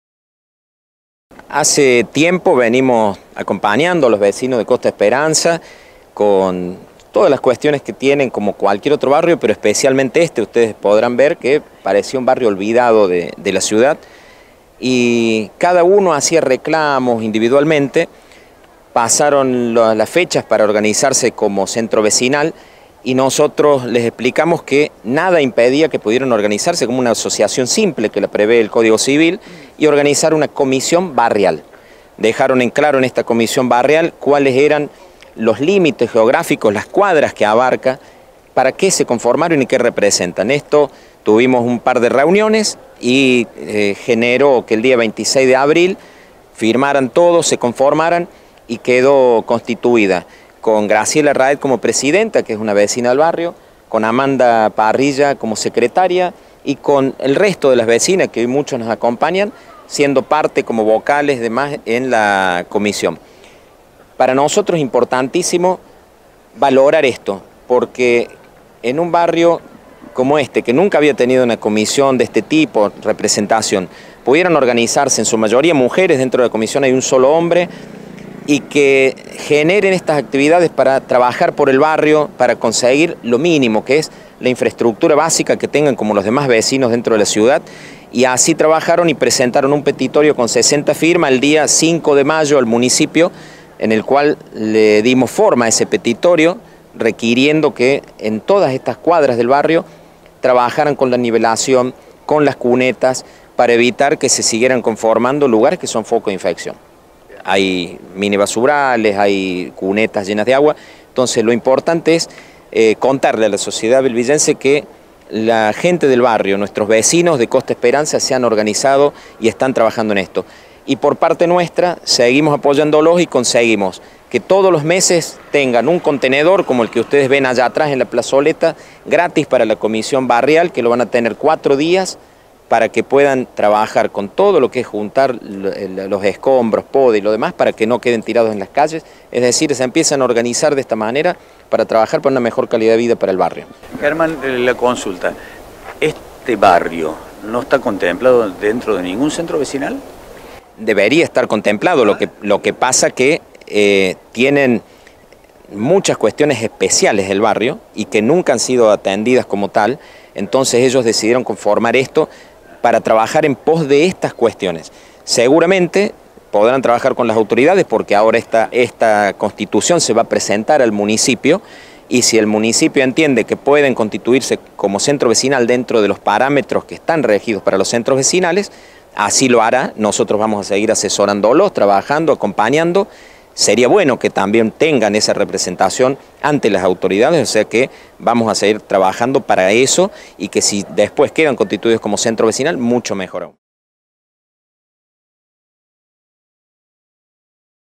VECINOS DEL BARRIO COSTA ESPERANZA BUSCAN SER RECONOCIDOS COMO CENTRO VECINAL (1º)
Recibieron asesoramiento de Germán Villarroel, dirigente loca, para la conformación de una comisión barrial.